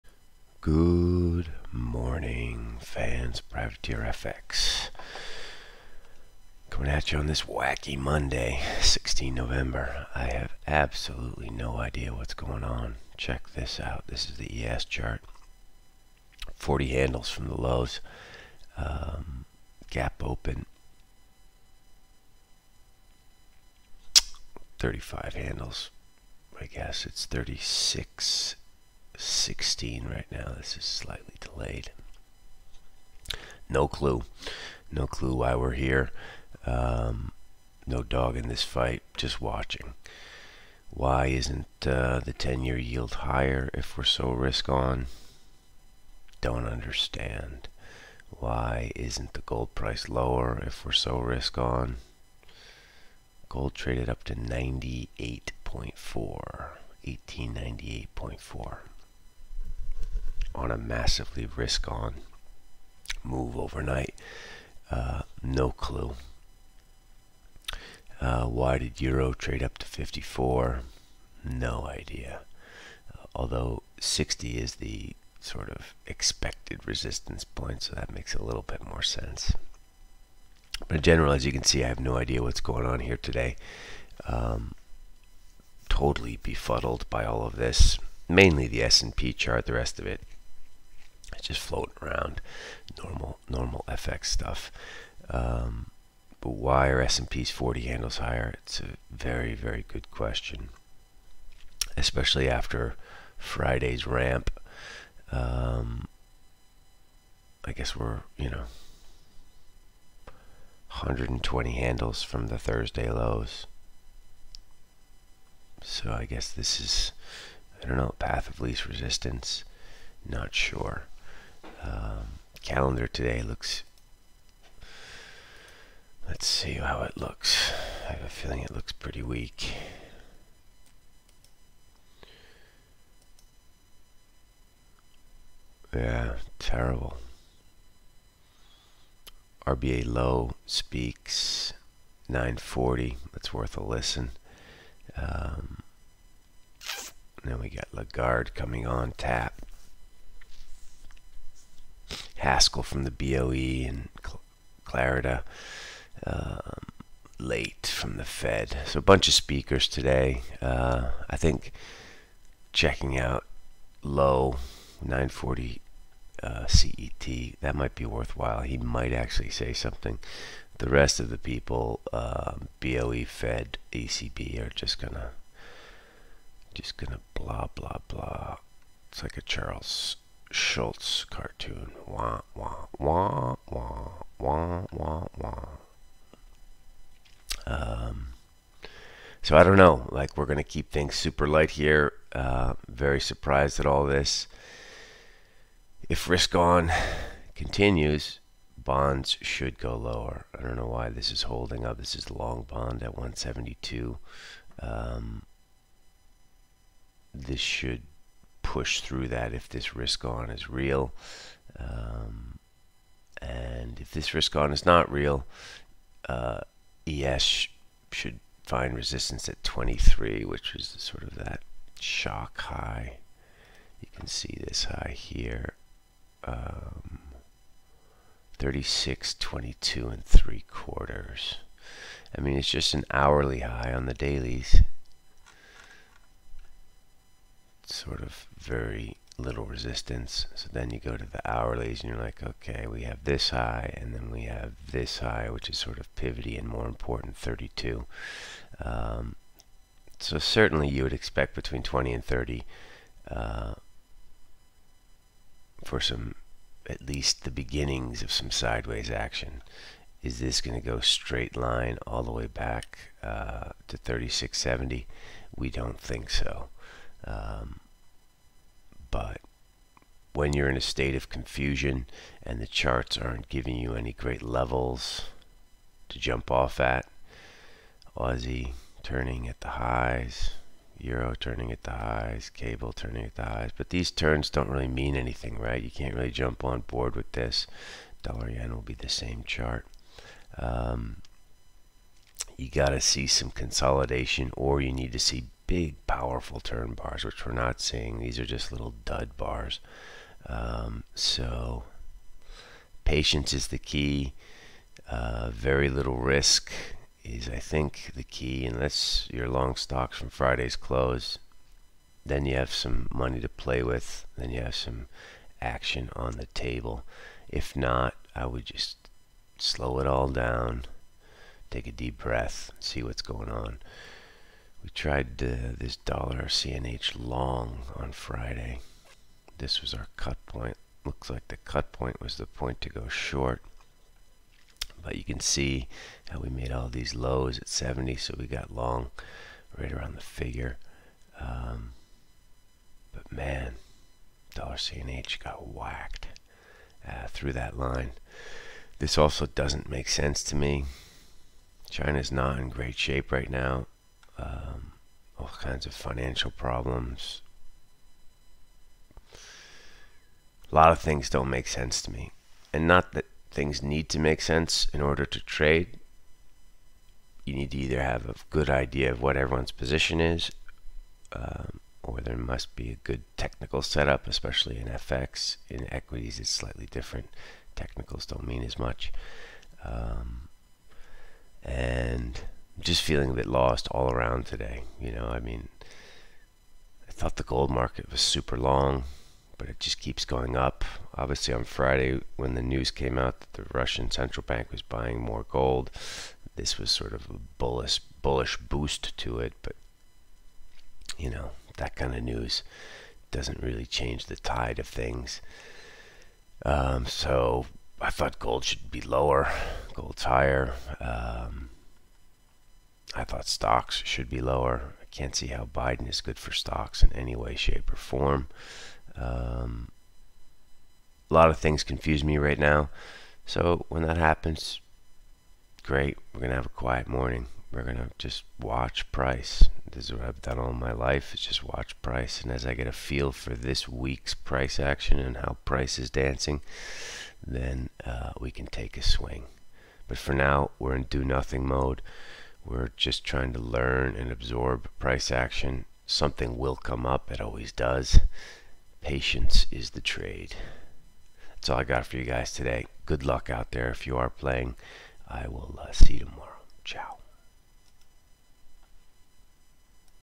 FOREX TRADING Morning Call 16 Nov (AUDUSD, EURUSD, GBPUSD) institutional view for individual traders
Risk on in SPX/ES /ES_F up 120 handles from Thurs lows ... not clear what's going on with bonds & Gold also up

Web: PrivateerFX.com
You can help keep this channel afloat (e.g. covering website hosting fees & beer) by clicking on this donations